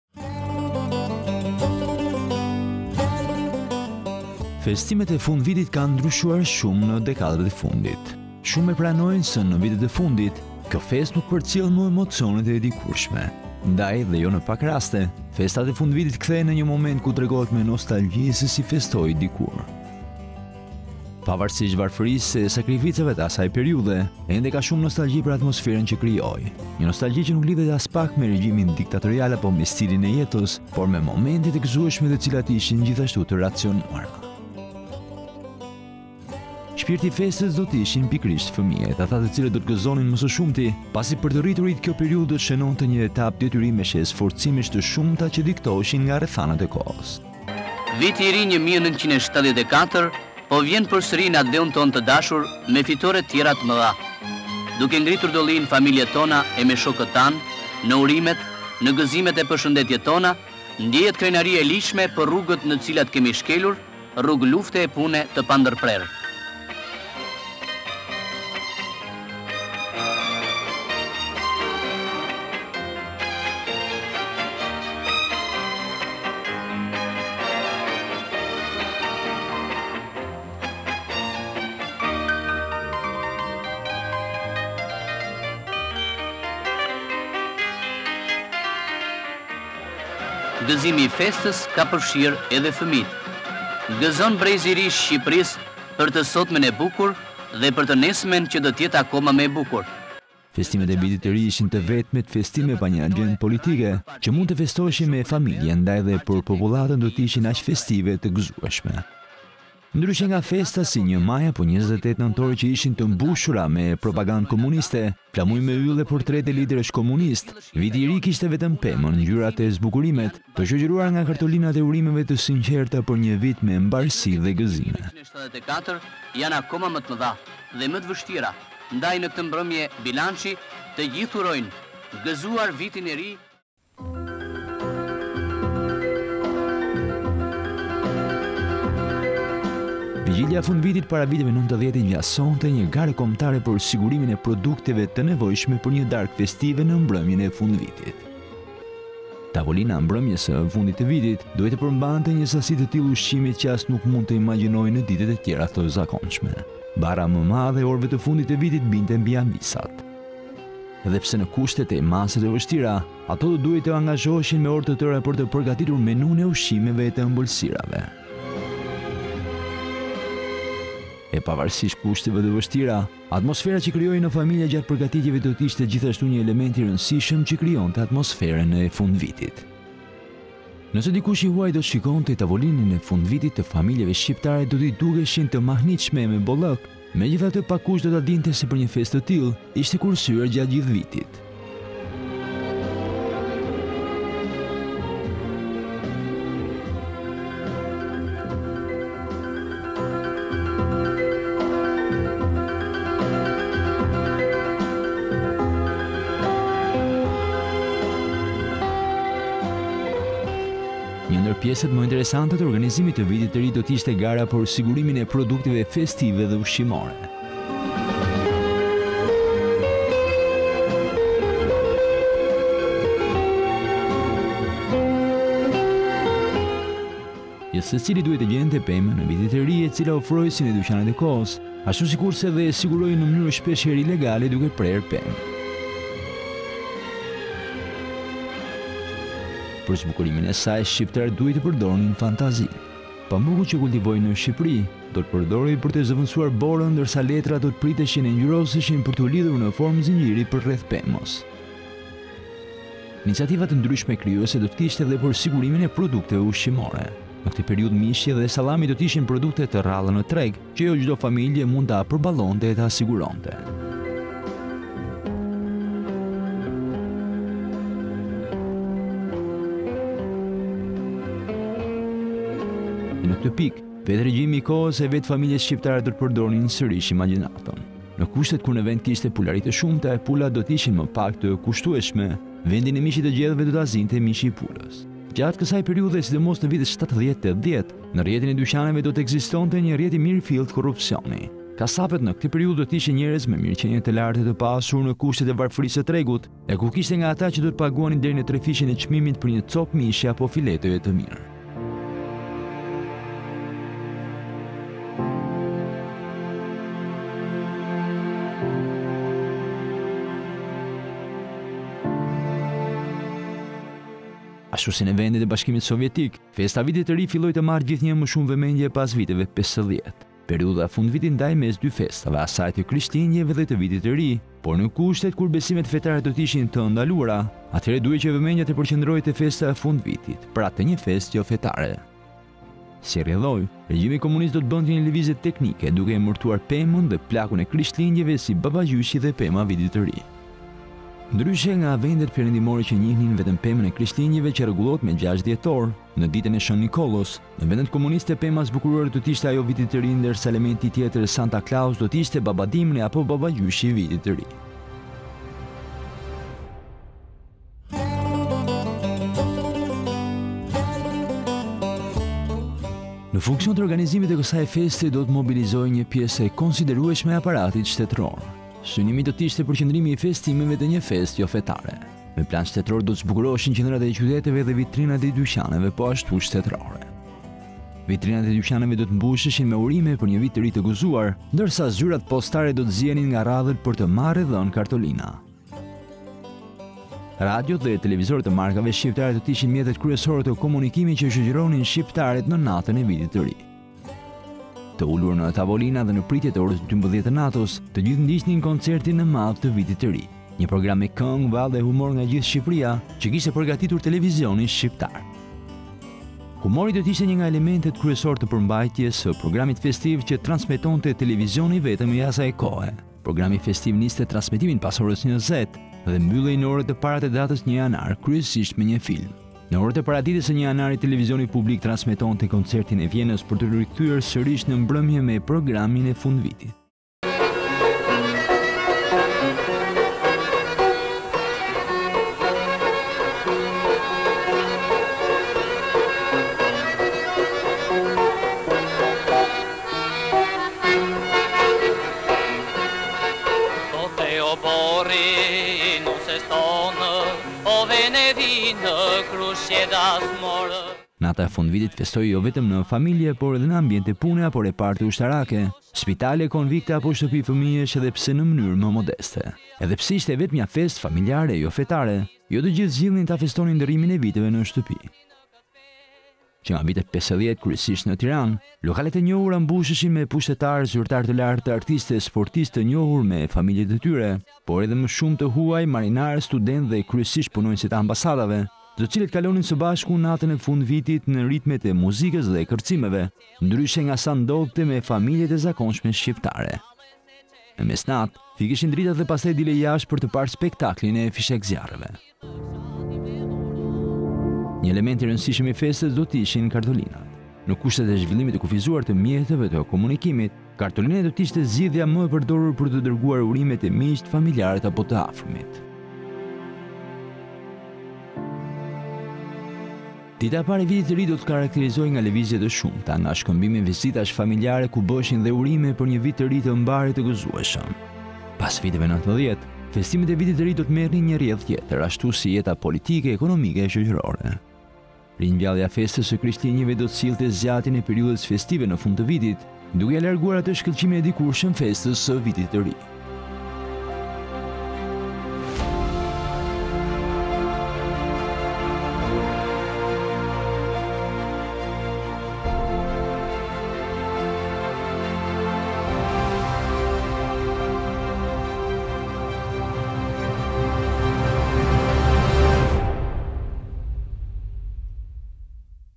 Si festohej Viti i Ri në komunizëm. E vetmja feste pa simbole partiake e fetare| DOSJA E
Festimet e Vitit të Ri ishin të vetmet festime pa një axhendë politike, që mund të festoheshin me familjen,  ndaj edhe për popullatën do të ishin aq festive e të gëzueshme.
Ndryshe nga festa si 1 maji apo 28 nëntori që ishin të mbushura me propagandë komuniste, flamuj me yll e portrete liderësh komunistë, Viti i Ri kishte vetëm pemën, ngjyrat e zbukurimet, të shoqëruara nga kartolinat e urimeve të sinqerta për një vit me mbarësi e gëzime.
Vigjilja e fundvitit para viteve ’90 i ngjasonte një gare kombëtare për sigurimin e produkteve të nevojshme për një darkë festive në mbrëmjen e fundvitit.
Tavolina e mbrëmjes së fundit të vitit duhej të përmbante një sasi të tillë ushqimi që as nuk mund të imagjinohej në ditët e tjera të zakonshme.
Barra më e madhe orëve të fundit të vitit binte mbi amvisat. Edhe pse në kushte tej mase të vështira, ato do të duhej të angazhoheshin me orë të tëra për të përgatitur menunë e ushqimeve e të ëmbëlsirave.